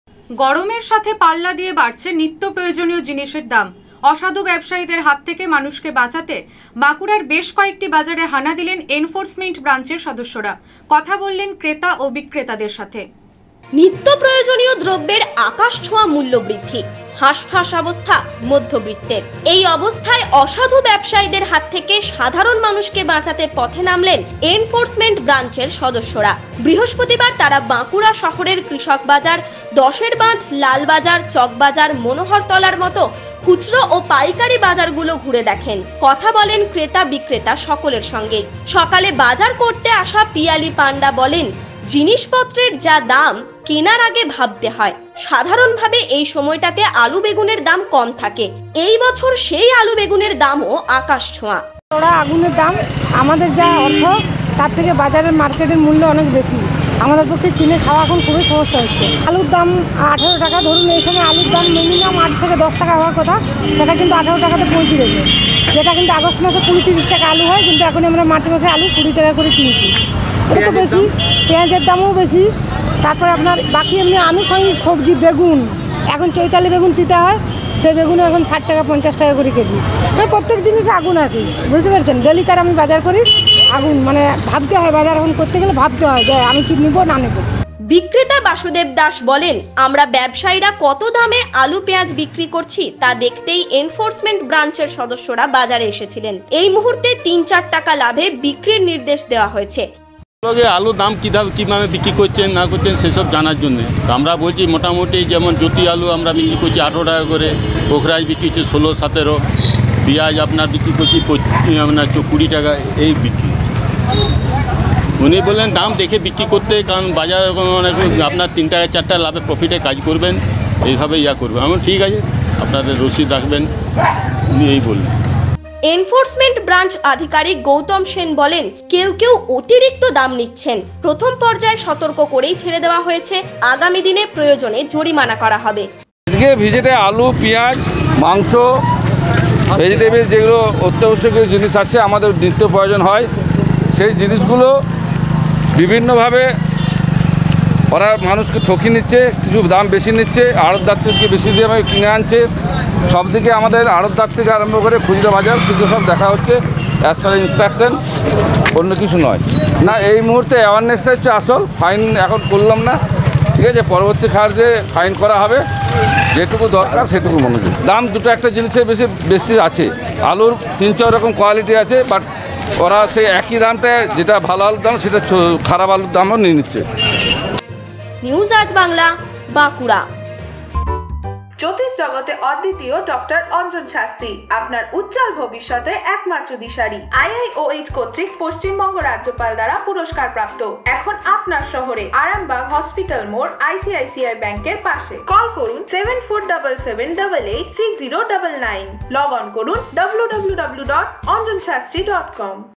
মূল্যবৃদ্ধি, খতিয়ে দেখতে বাজারে এনফোর্সমেন্ট বাঞ্চ
মুখ্যমন্ত্রীর নির্দেশ মতোই বাঁকুড়ার বিভিন্ন বাজার ঘুরে মূল্যবৃদ্ধির পরিমাণ খতিয়ে দেখলেন এনফোর্সমেন্ট ব্রাঞ্চের সদস্য